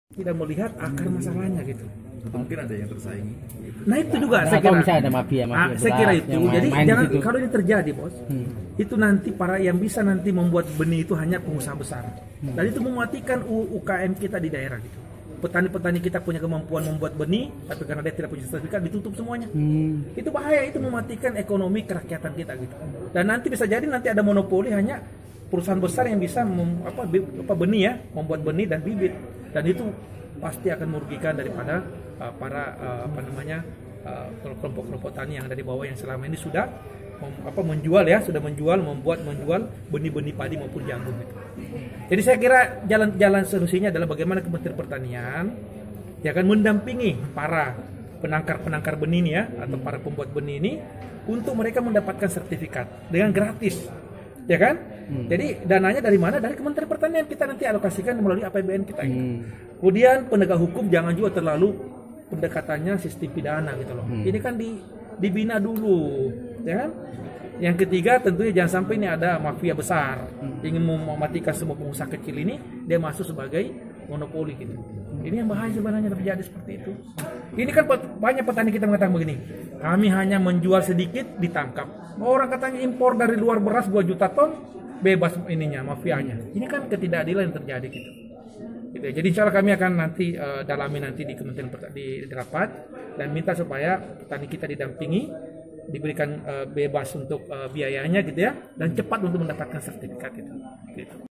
Sikapi Kasus Munirwan , Anggota DPR: Jangan Sampai Ada Mafia Besar Mematikan Pengusaha kecil
Anggota Komisi IV DPR RI, Andi Akmal Pasluddin angkat suara terkait kasus yang menimpa Tgk. Munirwan, kepala desa (Keuchik) Meunasah Rayeuk, Kecamatan Nisam, Kabupaten Aceh Utara.

Jangan Sampai Ada Mafia Besar Mematikan Pengusaha kecil

Dia melihat kasus ini seharunya dilihat dari akar permasalahannya terlebih dahulu. Dalam hal ini, Andi juga menyinggung masalah benih padi bersertifikat.

 "nanti bisa jadi yang bisa membuat benih hanya pengusaha besar", Andi Akmal Pasluddin.